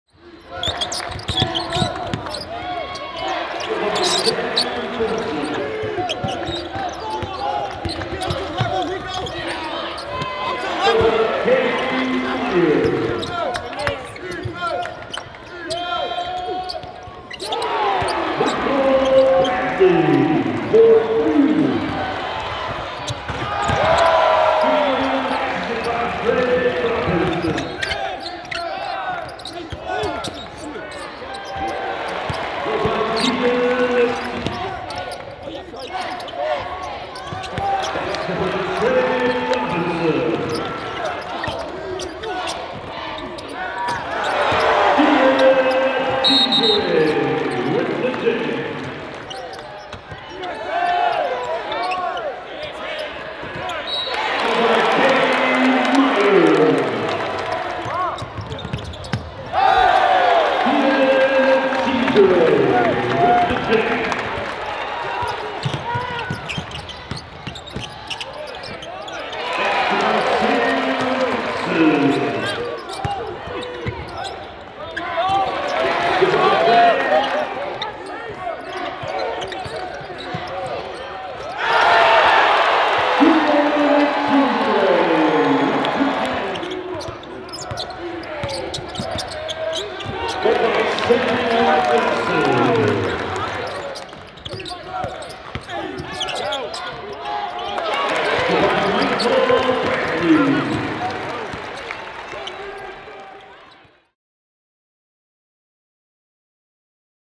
NKU MBB Highlights: NKU 72 - LIU 64
Highlights from Northern Kentucky men's basketball's 72-64 win over LIU on Saturday afternoon inside Truist Arena. The Norse went a perfect 2-0 to win the holiday tournament in Highland Heights.